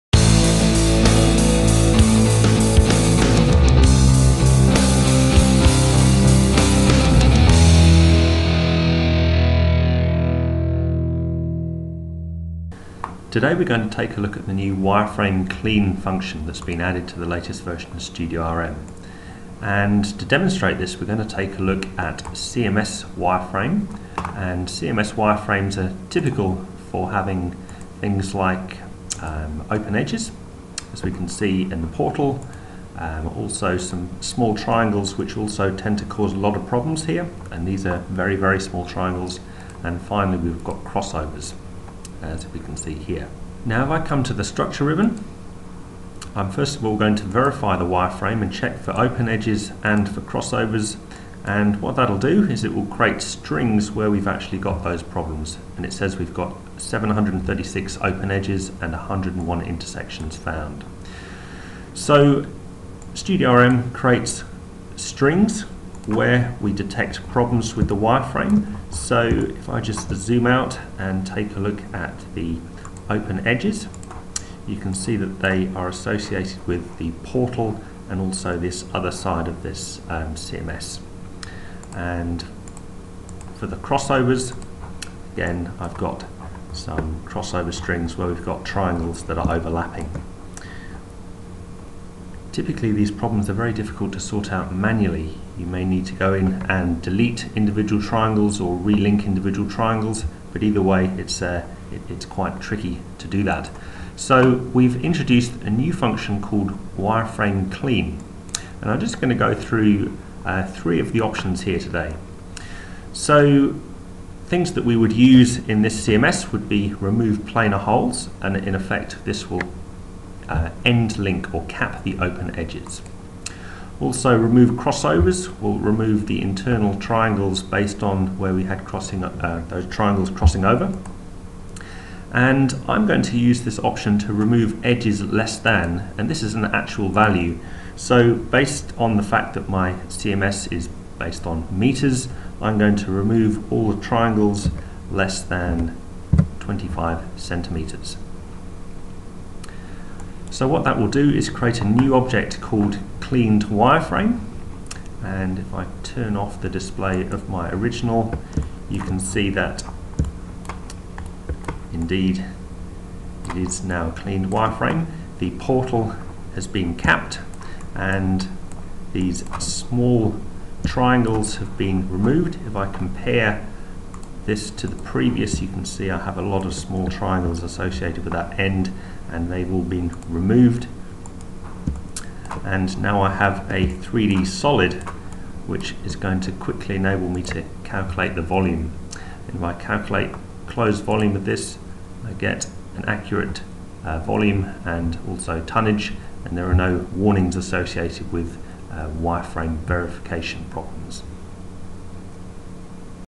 Datamine Studio - Automatic Wireframe Fix
Using the new 'Wireframe Clean' function in Datamine Studio to fix common problems with wireframes such as open edges and crossovers.